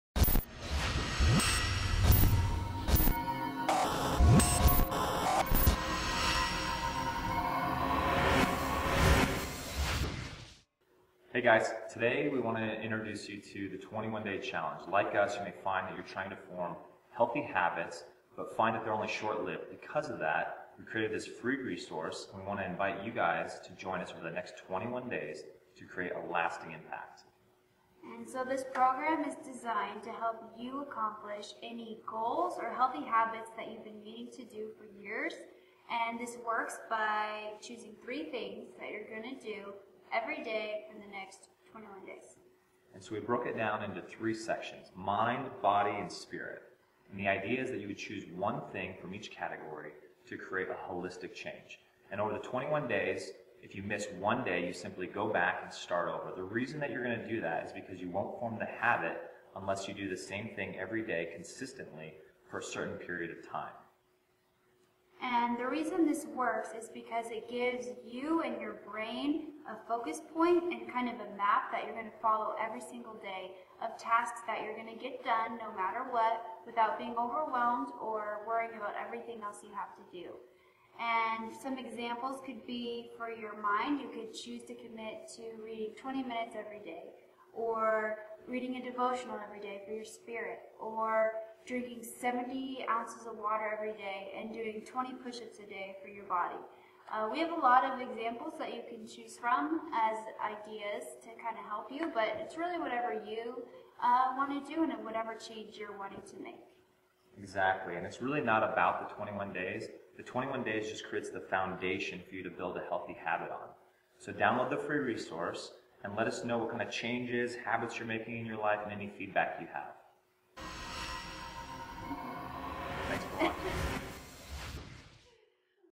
Renewing 21 Day Challenge
Simply choose 3-5 things you will commit to doing every day, for the next 21 days.

This is amazing because it gives you a focus point for each day and allows you to create habits and accomplish goals you have been meaning to do for years.

We suggest choosing things that nourish your mind, body and spirit

At the end of 21 days you could healthier, lose weight, hear from God, create a project, sharpen your mind, have more energy, create lasting habits. Stronger mind, body and spirit.